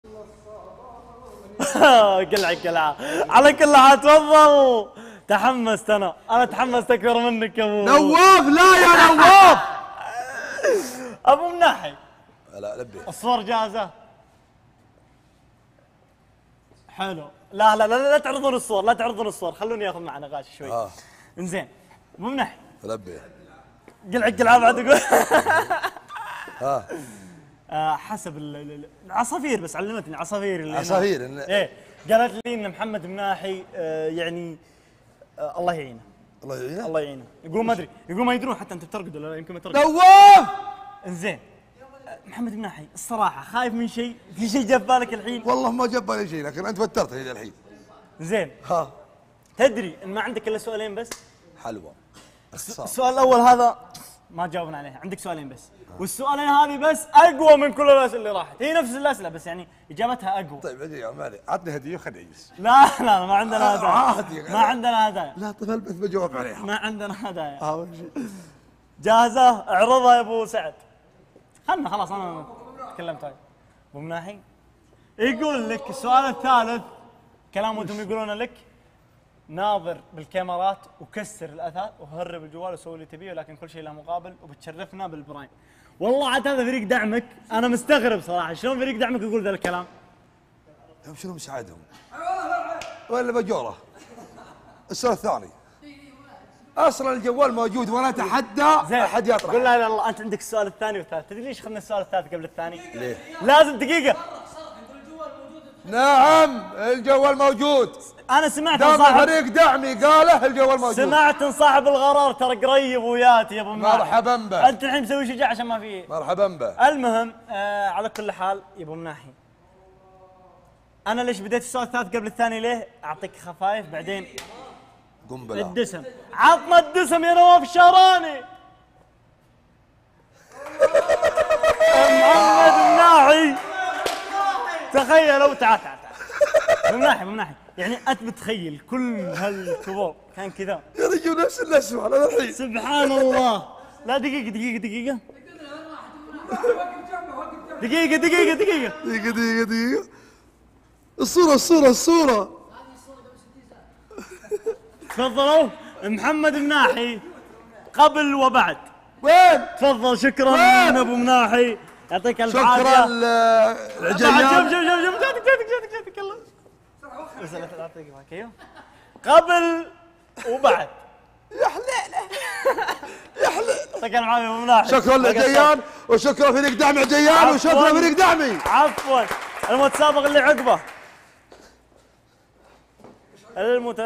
محمد مناحي يحاول تقمّص شخصيته في طفولته ! | فعالية عجيان المري
تابعنا على شبكات التواصل الإجتماعي:
ــــــــــــــــــــــــــــــــــــــــــــــــــــــــــــــــــ